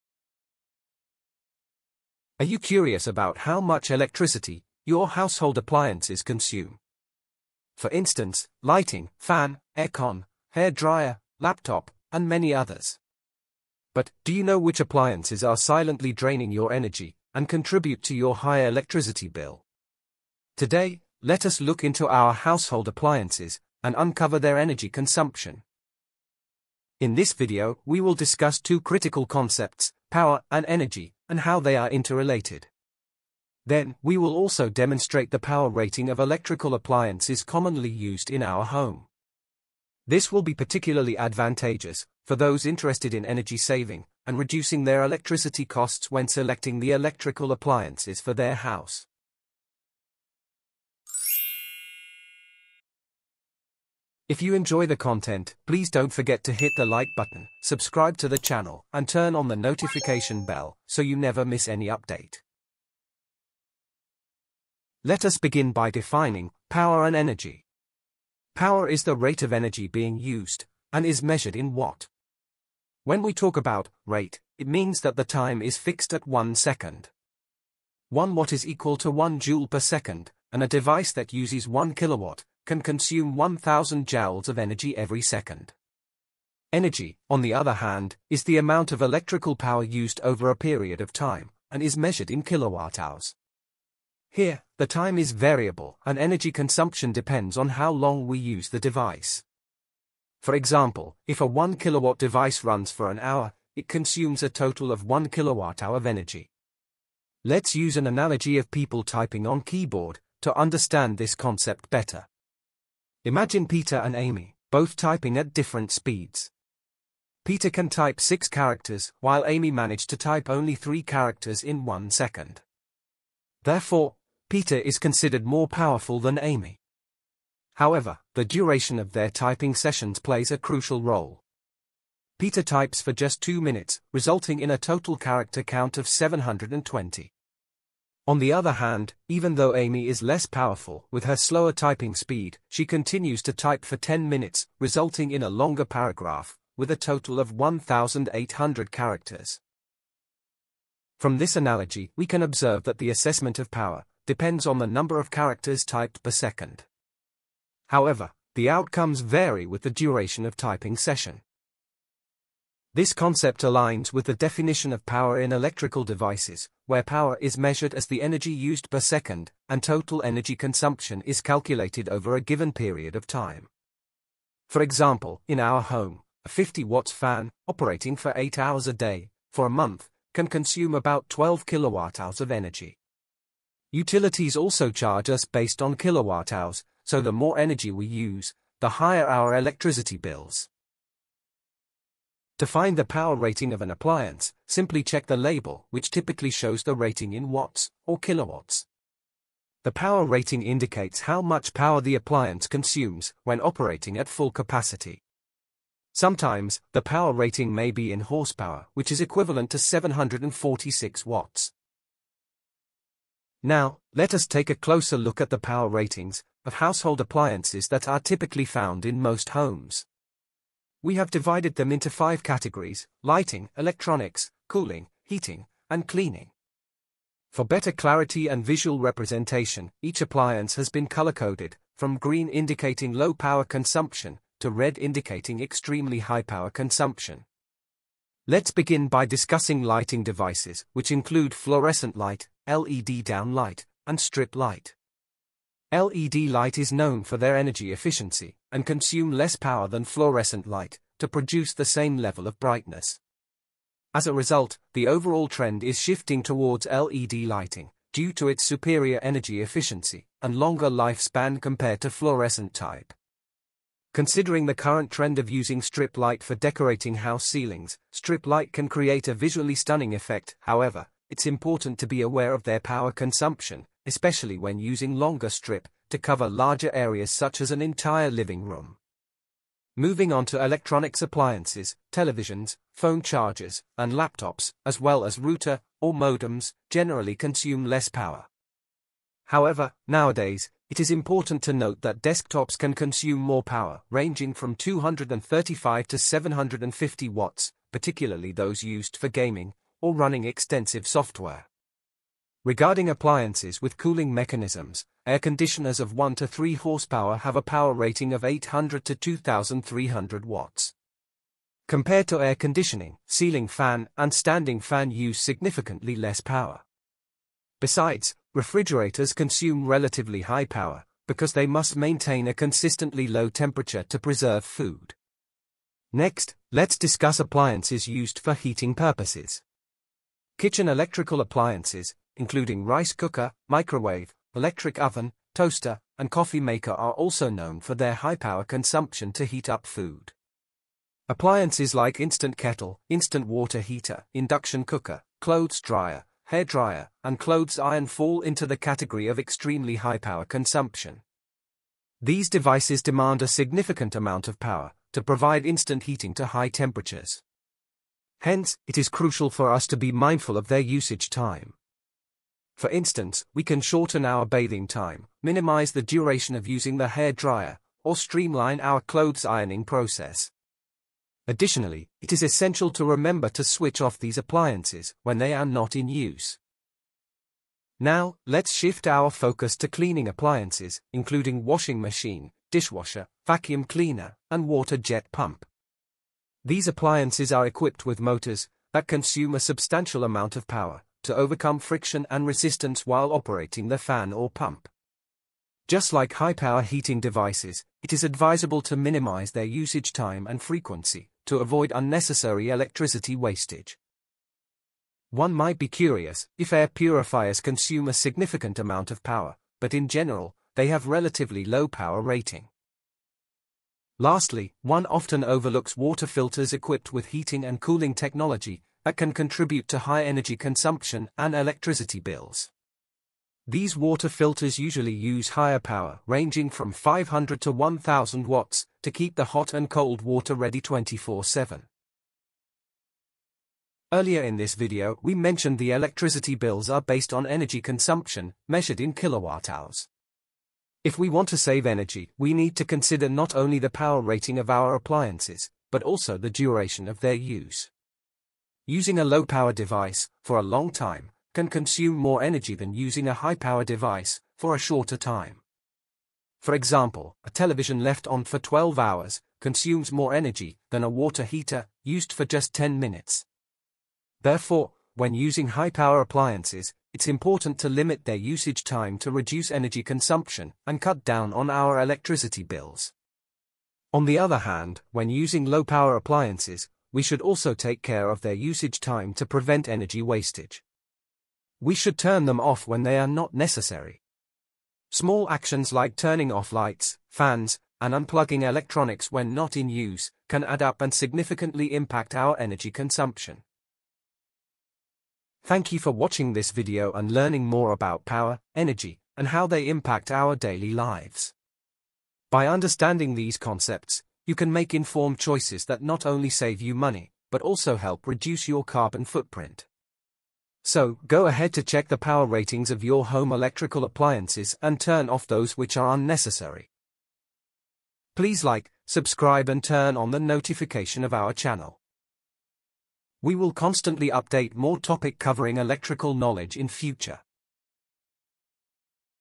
Household Appliances with HIGH Power Consumption | Electricity bill | Power vs Energy
Have you ever wondered how much electricity your household appliances consume? Get ready to discover the hidden truth behind the power ratings of common appliances used in your everyday life.

Join us as we delve into the fascinating world of power consumption. We'll explore the energy requirements of various appliances, from lighting devices to electronics, cooling systems to heating appliances, and cleaning equipment. You'll be amazed to learn which appliances silently drain your energy and contribute to those dreaded high electricity bills.

Don't miss out on this informative and engaging video that will transform the way you see your household appliances.